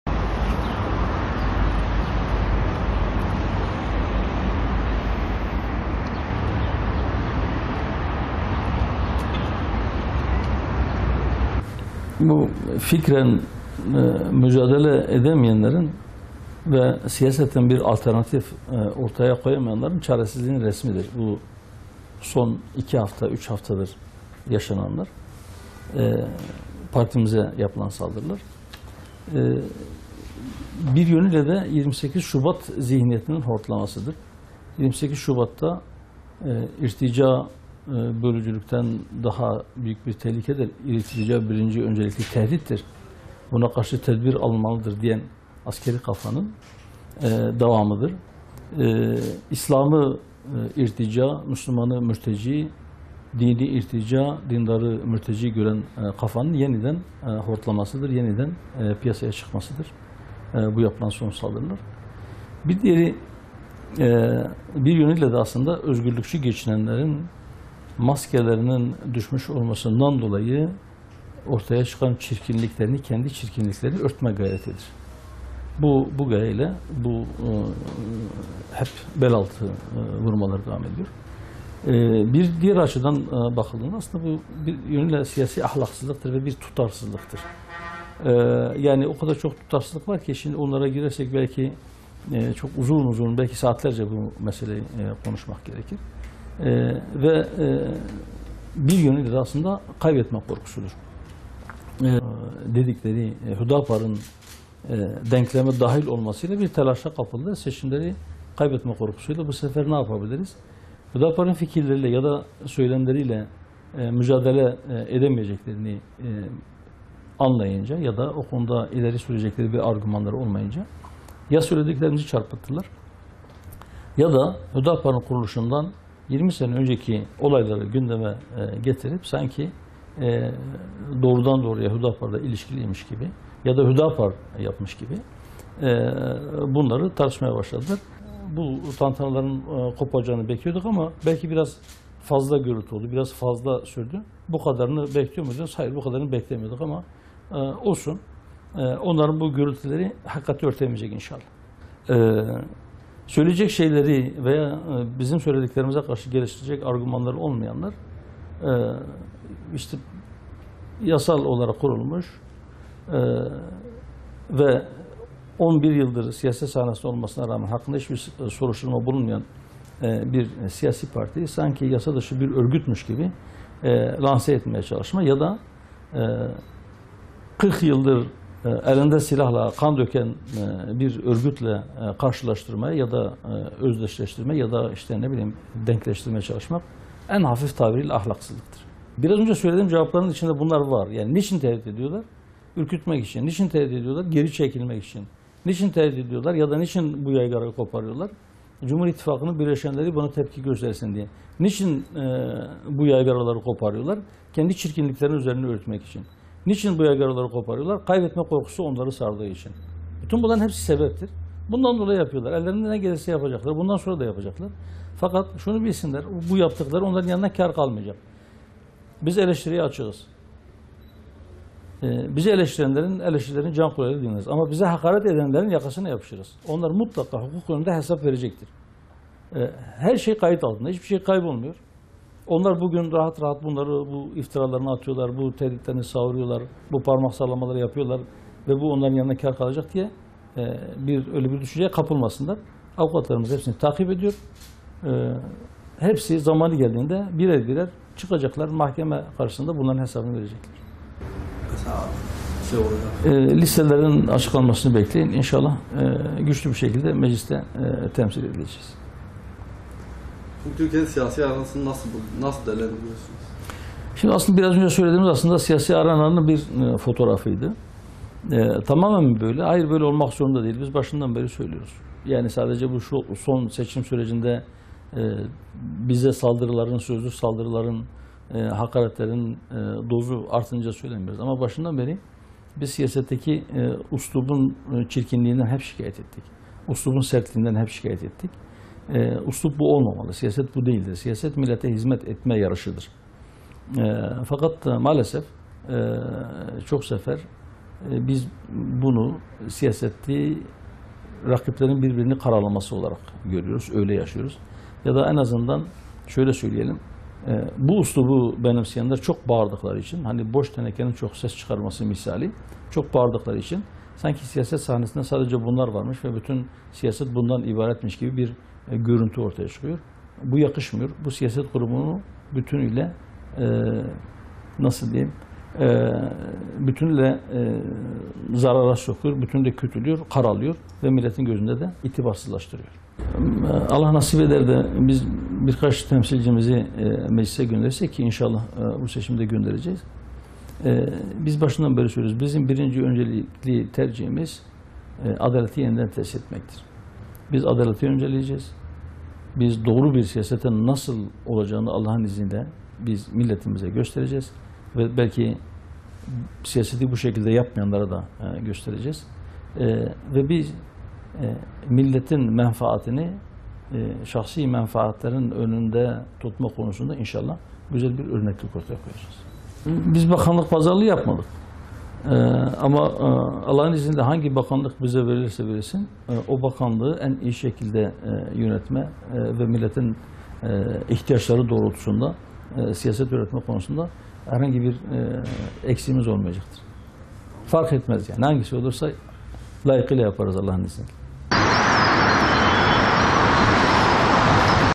Yapıcıoğlu: Hüda Par'ı, 40 Yıldır Kan Döken Örgütle Denkleştirmeye Çalışmak Ahlaksızlıktır
Hür Dava Partisi (HÜDA PAR) Genel Başkanı Zekeriya Yapıcıoğlu, “Yasal olarak kurulmuş ve 11 yıldır siyaset sahnesinde olmasına rağmen hakkında hiçbir soruşturma bulunmayan bir siyasi partiyi sanki yasa dışı bir örgütmüş gibi lanse etmeye çalışma ya da 40 yıldır elinde silahla kan döken bir örgütle karşılaştırma, özdeşleştirme, denkleştirmeye çalışmak; en hafif tabiriyle ahlaksızlıktır” dedi.
Diyarbakır’a gelen HÜDA PAR Genel Başkanı Zekeriya Yapıcıoğlu, Cumhur İttifakı adayı Cumhurbaşkanı Recep Tayyip Erdoğan’ı destekleyeceklerini açıklamalarının ardından bazı kesimlerce tehdit edilip, sözlü saldırılara uğradıklarını söyledi. Yapıcıoğlu, “Bize karşı saldırılar, fikren mücadele edemeyenlerin, siyaseten bir alternatif koyamayanların çaresizliğinin resmidir. Son 2-3 haftadır partimize yapılan saldırılar böyledir. Bir yönüyle de 28 Şubat zihniyetinin hortlamasıdır. Bu yapılan saldırıların İslam’ı irtica, Müslüman’ı mülteci gören kafanın yeniden hortlamasıdır; piyasaya çıkmasıdır. Özgürlükçü geçinenlerin maskelerinin düşmüş olmasından dolayı ortaya çıkan kendi çirkinliklerini örtme gayretidir. Bu gaye ile bize hep bel altı vurmalar devam ediyor. Bir diğer yanı ile bakıldığında; bu siyasi ahlaksızlıktır, bir tutarsızlıktır. Seçimleri kaybetme korkusuyla bu sefer, ‘Ne yapabiliriz’ düşüncesi ile ya söylediklerimizi çarpıttılar ya da HÜDA PAR’ın kuruluşundan 20 yıl önceki olayları gündeme getirip, sanki doğrudan HÜDA PAR ile ilişkiliymiş gibi veya HÜDA PAR yapmış gibi bunları tartışmaya başladılar. Bu tantanaların kopacağını bekliyorduk ama biraz fazla sürdü. Bu kadarını biz de beklemiyorduk. Bu gürültüler, hakikatleri örtemeyecektir” dedi.
‘BİZE HAKARET EDENLERİN YAKASINA YAPIŞACAĞIZ’
HÜDA PAR’ın yasal bir parti olduğunu, hakkında herhangi bir soruşturma olmamasına rağmen kendilerine iftira ve hakaret edenlerin hukuk önünde hesap vereceğini belirten Yapıcıoğlu, “Yasal olarak kurulmuş ve 11 yıldır siyaset sahnesinde olmasına rağmen hakkında hiçbir soruşturma bulunmayan bir siyasi partiyi sanki yasa dışı bir örgütmüş gibi lanse etmeye çalışma ya da 40 yıldır elinde silahla kan döken bir örgütle karşılaştırma, özdeşleştirme, denkleştirmeye çalışmak; en hafif tabiriyle ahlaksızlıktır. Bizi neden tehdit ediyorlar? Bu yaygarayı neden yapıyorlar? Cumhur İttifakı’nın bileşenleri, ‘Tepki göstersinler’ diye yapıyorlar.  Niçin? Kendi çirkinliklerinin üstünü kapatmak ve kaybetme korkuları onları sardığı için saldırıyorlar. Bütün bunların hepsi sebeptir. Ellerinden ne gelirse yapacaklar, bundan sonra da yapmaya devam edecekler. Fakat bunu bilsinler; bu yaptıkları yanlarına kar kalmayacak. Eleştiriye açığız. Bizi eleştirenleri, can kulağıyla dinleriz. Bize hakaret edenlerin yakasına yapışacağız. Onlar mutlaka hukuk önünde hesap verecektir. Her şey kayıt altında ve hiçbir şey kaybolmuyor. Bugün rahat rahat iftiralarını, tehditlerini, parmak sallamalarını yapıyorlar. Bunlar yanlarına kar kalacak düşüncesine kapılmasınlar. Avukatlarımız, hepsini takip ediyor. Bunlar, birer birer mahkeme karşısına çıkıp, hesap verecek” diye konuştu.
‘BAKANLIK PAZARLIĞI YAPMADIK’
Bakanlık pazarlığı yapmadıklarını ancak kendilerine verilecek herhangi bir bakanlığı da en iyi şekilde yöneteceklerini belirten Yapıcıoğlu, şunları söyledi:
“Listelerin açıklanmasını bekleyin. İnşallah güçlü bir şekilde Meclis’te temsil edileceğiz. Başından beri söylüyoruz; sadece son seçim döneminde bize sözlü saldırıların ve hakaretlerin dozu artınca söylemiyoruz. Ama başından bu yana siyasetteki üslubun, üslubun sertliğinden, çirkinliğinden hep şikayet ettik. Siyaset böyle olamaz. Siyaset; millete hizmet etme yarışıdır. Maalesef birçok defa siyaseti, rakiplerin birbirini karalaması olarak görüyoruz. Bu üslubu benimseyenler, çok bağırdıkları için boş tenekenin ses çıkarması misali; sanki siyaset sahnesinde varmış ve bütün siyaset bundan ibaretmiş gibi görüntü ortaya çıkıyor. Bu durum siyaseti bütünü ile zarara sokarak kötülüyor ve aynı zamanda da milletin gözünde itibarsızlaştırıyor. Birkaç temsilcimizi Meclis’e gönderirsek, inşallah göndereceğiz de. Bizim başından beri söylediğimiz adaleti yeniden tesis etmektir. Adaleti önceleyeceğiz. Doğru siyasetin nasıl olduğunu milletimize göstereceğiz. Siyaseti bu şekilde yapmayanlara da göstereceğiz. Milletin menfaatlerini, şahsi menfaatlerin önünde tutma konusunda da inşallah güzel bir örnek ortaya koyacağız. Bakanlık pazarlığı yapmadık. Ama hangi bakanlık bize verilirse verilsin; o bakanlığı en iyi şekilde yönetme ve milletin ihtiyaçları doğrultusunda siyaset öğretme konusunda herhangi bir eksiğimiz olmayacaktır.”